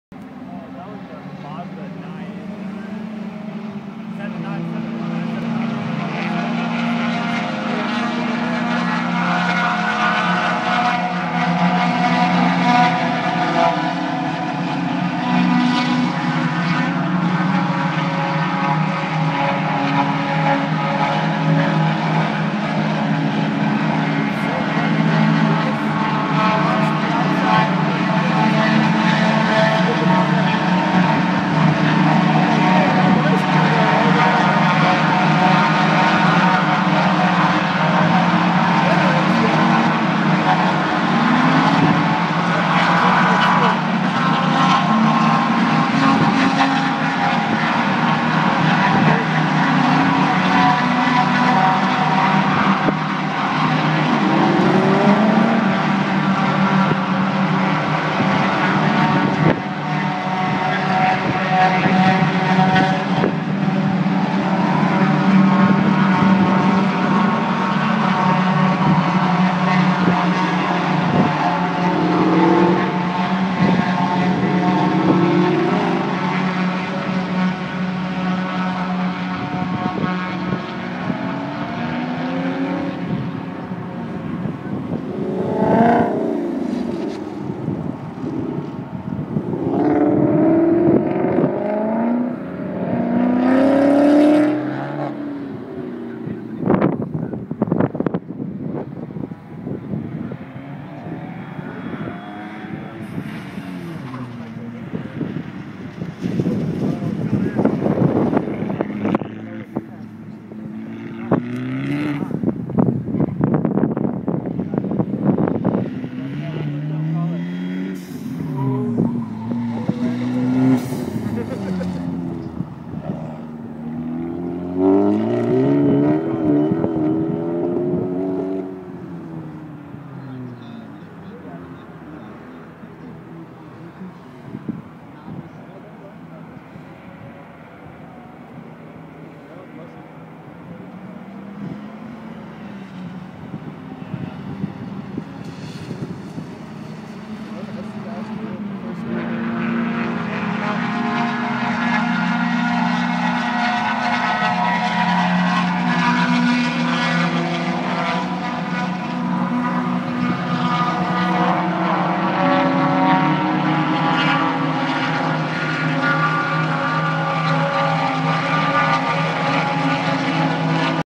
Sights and sounds of Daytona on 5/4/2014
SCCA Autocross during SCCA road racing.